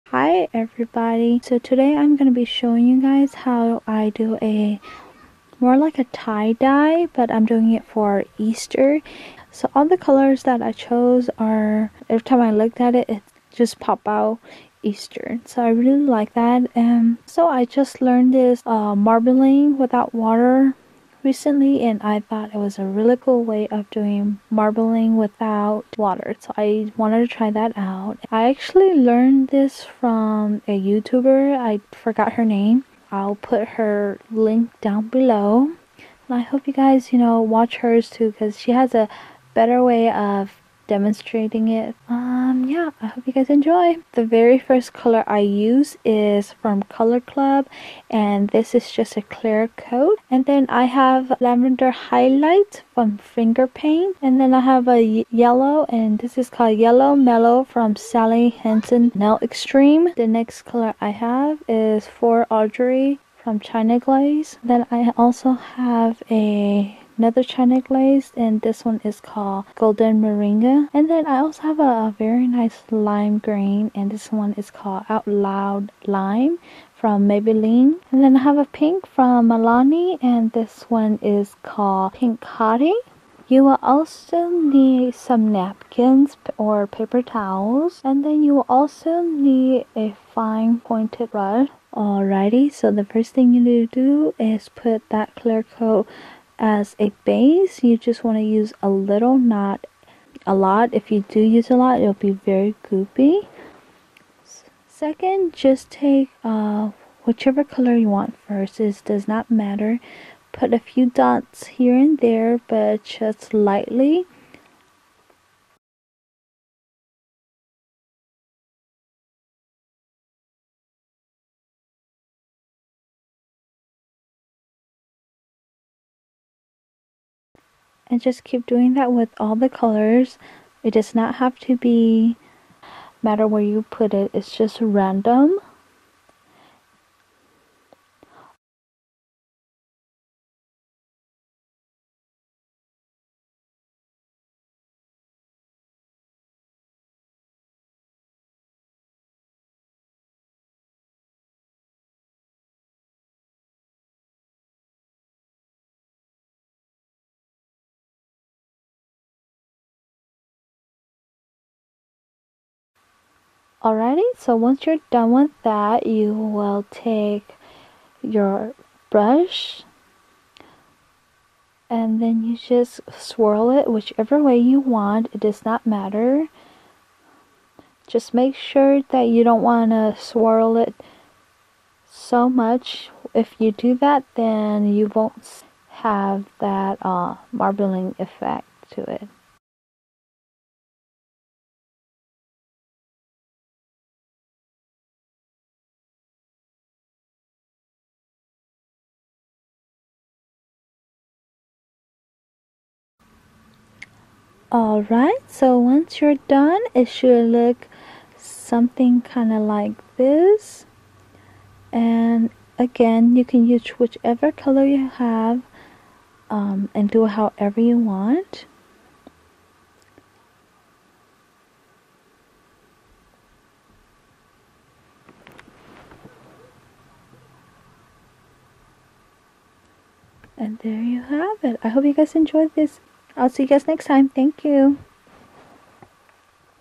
Easter Tie dye Marble Nail Art (with out water)
i did this the night before Easter Sunday. i hope you like this